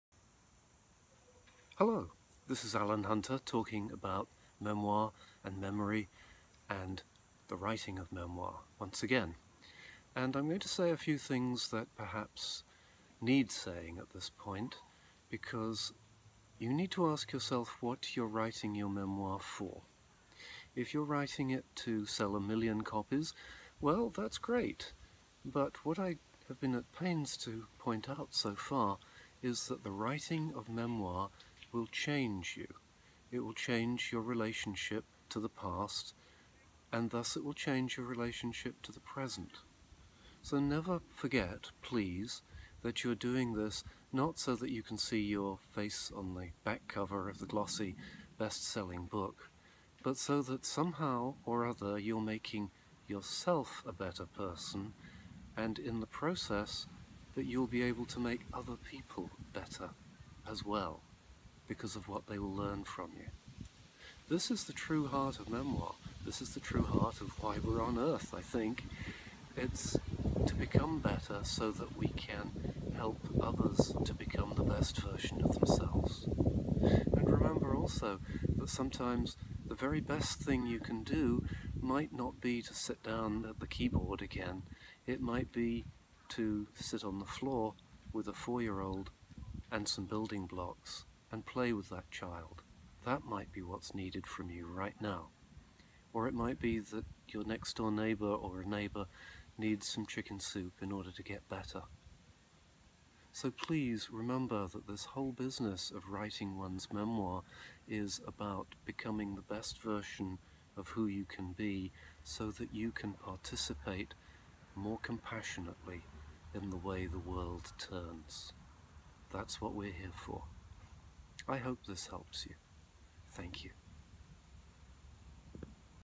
The true purpose of Memoir
Many writers put their memoirs together as a way of teaching others about what their life has shown them.  Sometimes writers hope to be famous. But sometimes there's a deeper reason - and it's one we must honor.